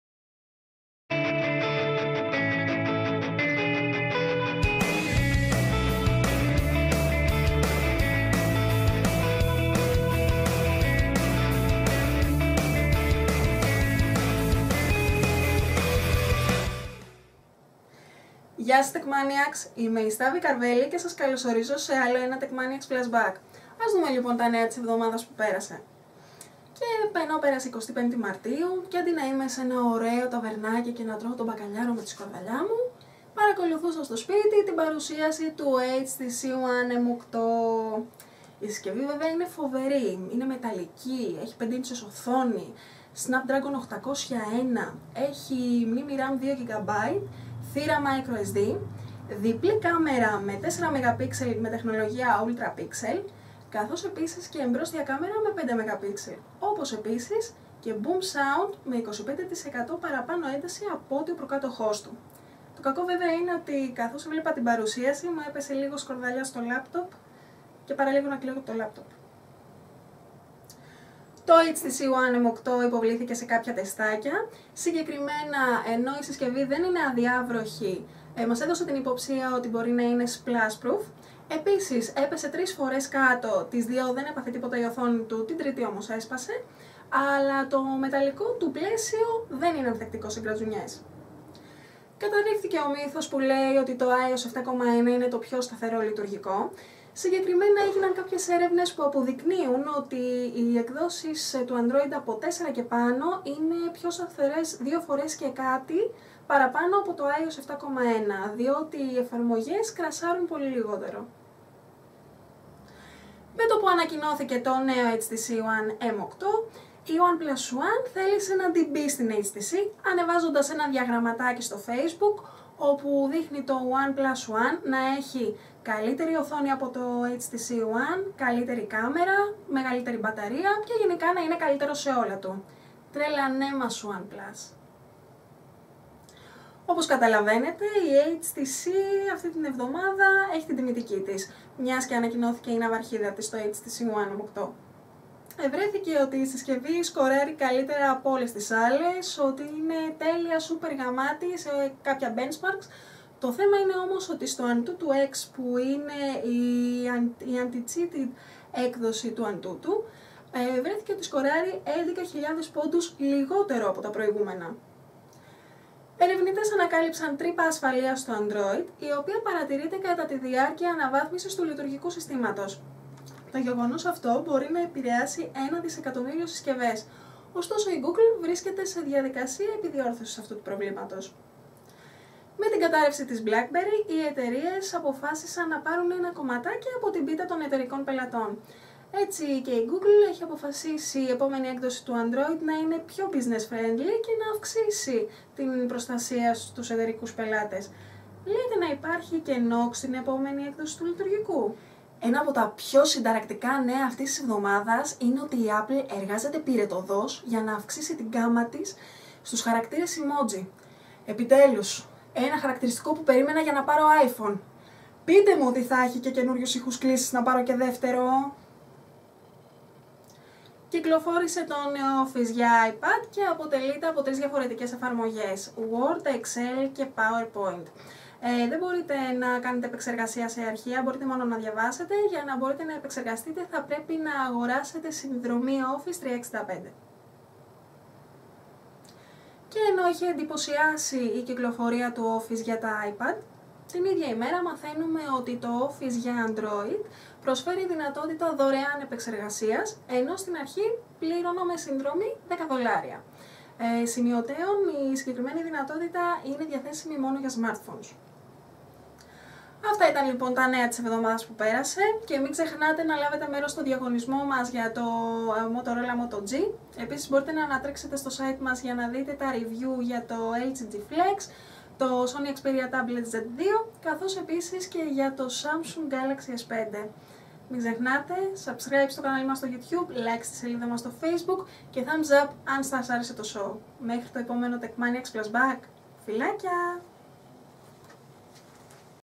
Techmaniacs Flashback ep. 16
Ανασκόπηση των τεχνολογικών νέων από την Σταύη των Techmaniacs για την εβδομάδα 24/3 με 30/3.